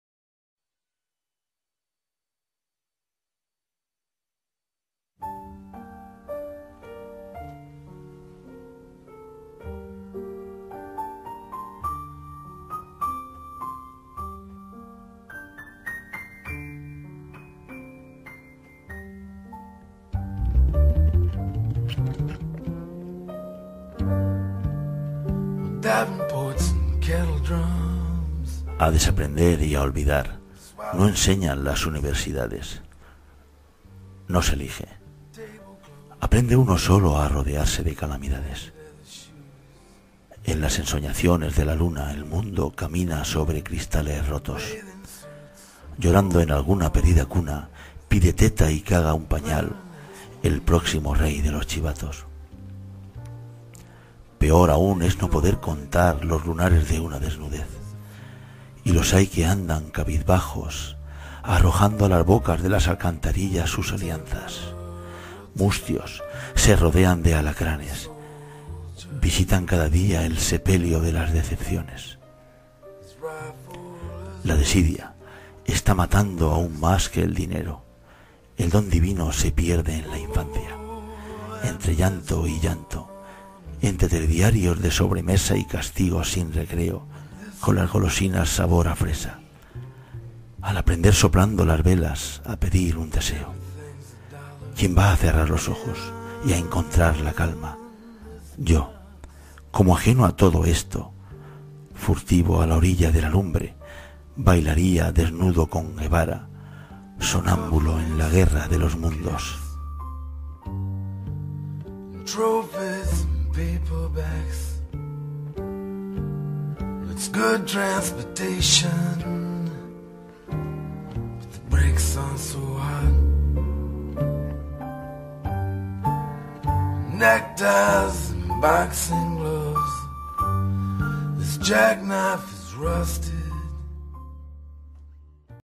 Bailaría con Guevara
Poema de Damián Varea recogido en el libro Entorno (Paseo poético) de Ed. Triquiñuela de artes y letras, 2006